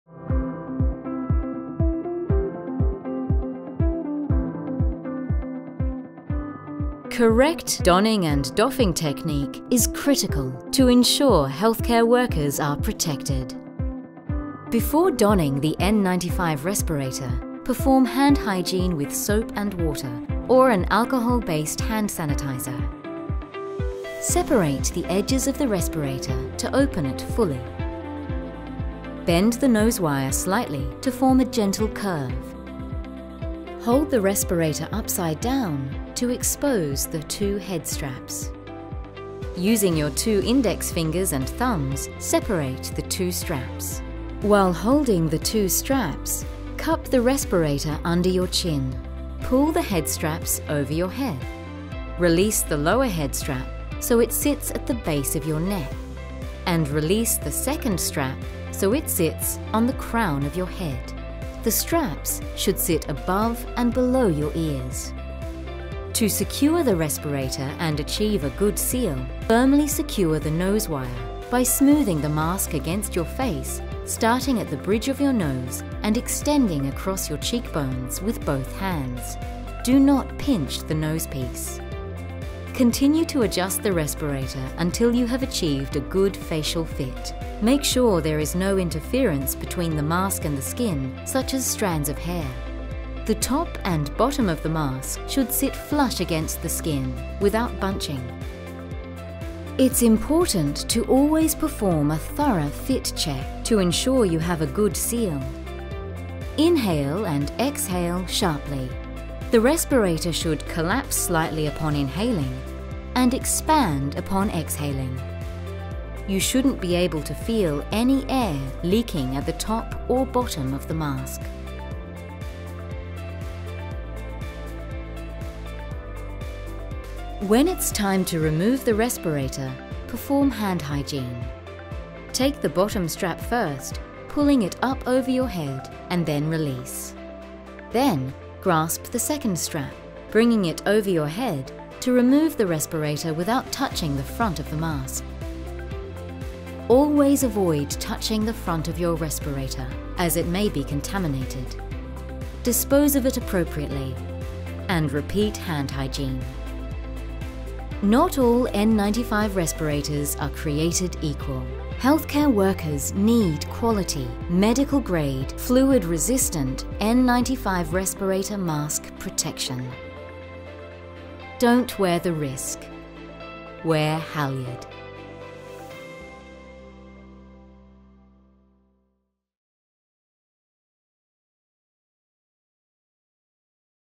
HALYARD FLUIDSHIELD N95 Respirator Donning & Doffing
Correct donning and doffing techniques are critical for Healthcare Worker protection.
Halyard’s NEW N95 Respirator Donning and Doffing Animation demonstrates how to correctly don (put on) and doff (remove) Halyard’s N95 Respirator through a series of steps.